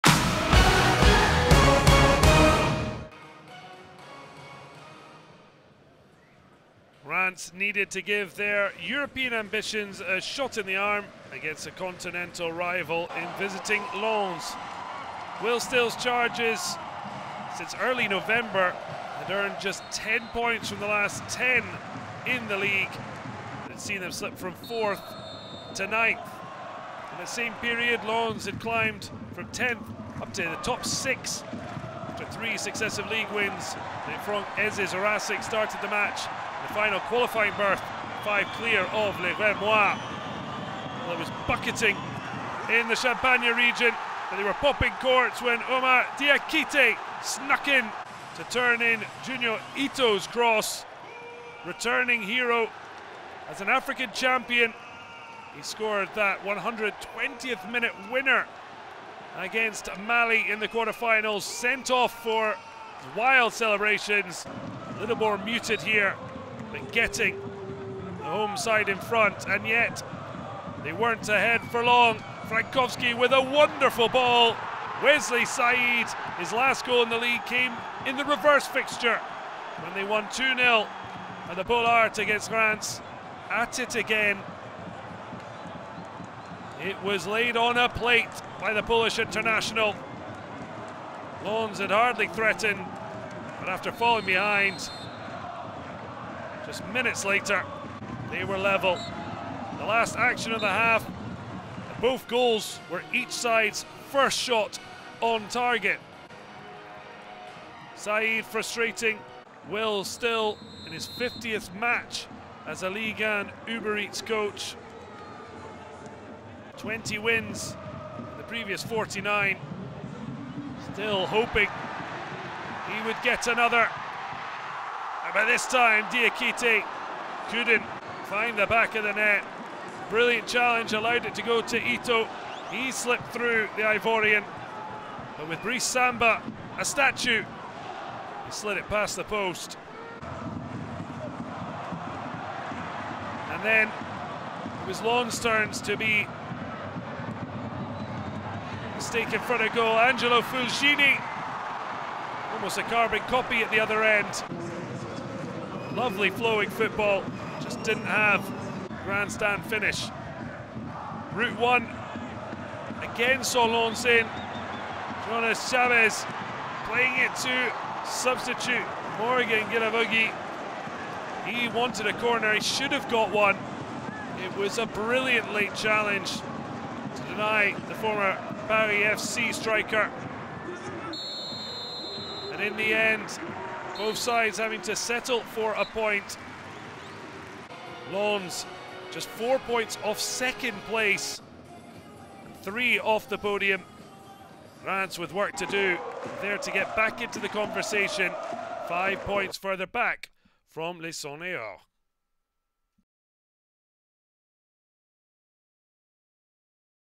STADE DE REIMS - RC LENS (1 - 1) - Résumé - (SdR - RCL) / 2023-2024
Revivez les meilleurs moments de STADE DE REIMS - RC LENS (1-1) en vidéo.

Ligue 1 Uber Eats - Saison 2023/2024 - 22ème journée STADE AUGUSTE-DELAUNE - dimanche 18 février 2024

Buteurs : Oumar DIAKITE (42' - SdR) / Wesley SAID (45' +1 - RCL)

Composition STADE DE REIMS : 5 - Yunis ABDELHAMID (c) /94 - Yehvann DIOUF /10 - Teddy TEUMA /32 - Thomas FOKET /15 - Marshall MUNETSI /24 - Emmanuel AGBADOU /7 - Junya ITO /14 - Réda KHADRA /22 - Oumar DIAKITE /2 - Joseph Stanley OKUMU /18 - Sergio AKIEME RODRIGUEZ

Composition RC LENS : 5 - Yunis ABDELHAMID (c) /94 - Yehvann DIOUF /10 - Teddy TEUMA /32 - Thomas FOKET /15 - Marshall MUNETSI /24 - Emmanuel AGBADOU /7 - Junya ITO /14 - Réda KHADRA /22 - Oumar DIAKITE /2 - Joseph Stanley OKUMU /18 - Sergio AKIEME RODRIGUEZ